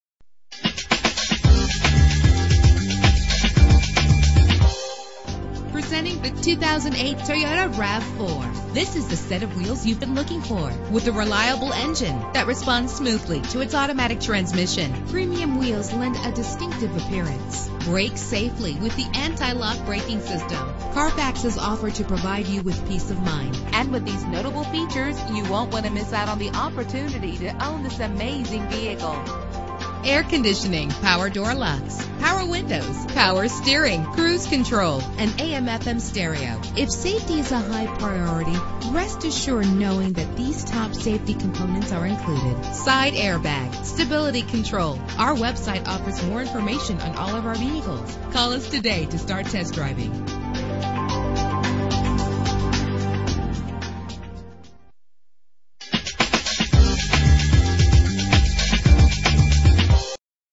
2008 Toyota RAV4 - Brockton MA
Year: 2008
Make: Toyota
Model: RAV4
Trim: Sport Sport Utility 4D
Engine: 4 cylinder 2.4 Liter
Transmission:
Color: Gray
Mileage: 25712
Address:
970 West Chestnut Street